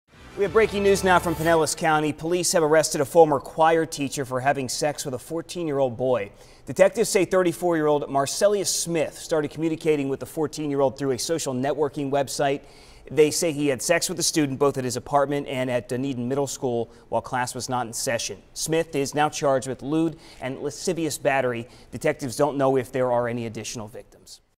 Former middle school chorus teacher arrested for having sex with 14-year-old student
A former Dunedin Middle School teacher has been arrested and charged for having a sexual relationship with a 14-year-old student.